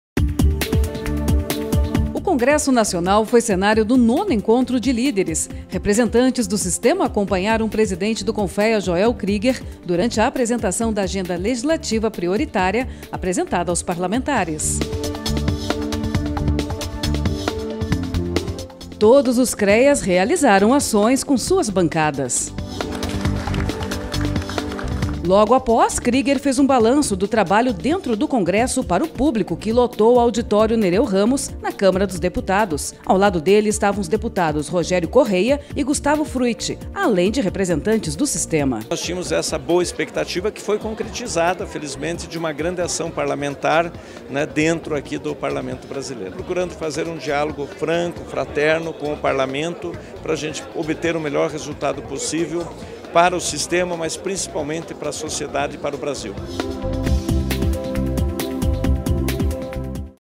9º Encontro de Líderes - Congresso Nacional
O Congresso Nacional também foi cenário do 9º Encontro de Líderes do Sistema. Assista e veja como foi!